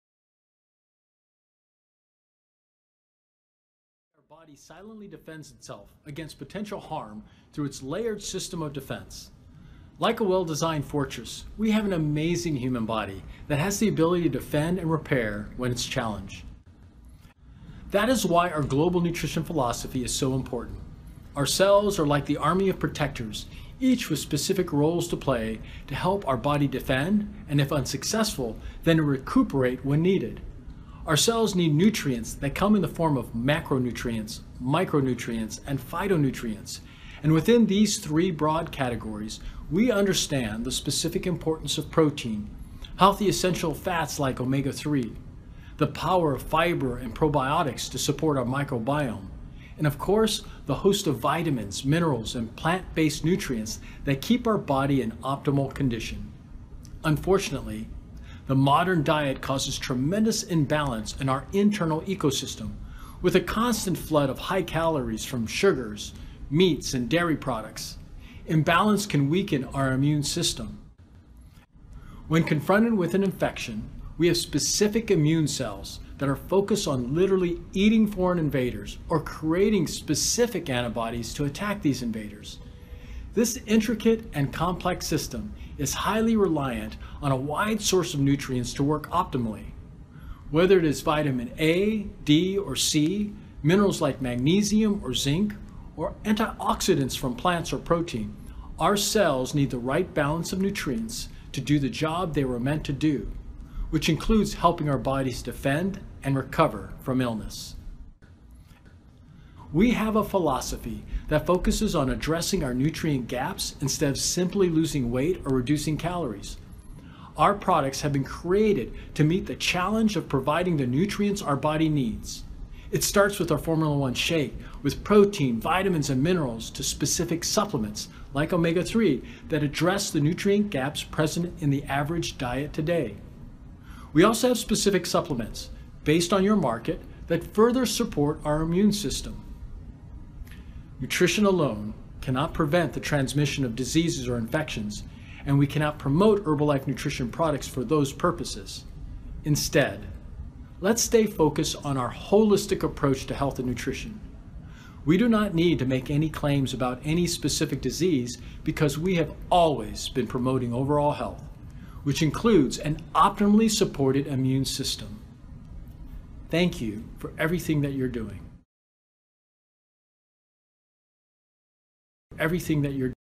Tự bảo vệ chính mình, với hệ miễn dịch khỏe mạnh
Mỗi ngày cơ thể chúng ta âm thầm tự bảo vệ mình trước những tác nhân gây hại thông qua nhiều lớp phòng thủ của hệ miễn dịch. Mỗi tế bào của cơ thể có vai trò riêng, tạo nên khả năng tự vệ của cơ thể. Vậy chúng ta cần làm gì để nuôi dưỡng tế bào và giúp cơ thể duy trì trạng thái tối ưu?
Hãy cùng lắng nghe những chia sẻ của Kent Bradley (M.D, MPH, MBA)
Giám Đốc phụ trách Sức Khỏe và Dinh Dưỡng Tập Đoàn Herbalife Nutrition nhé!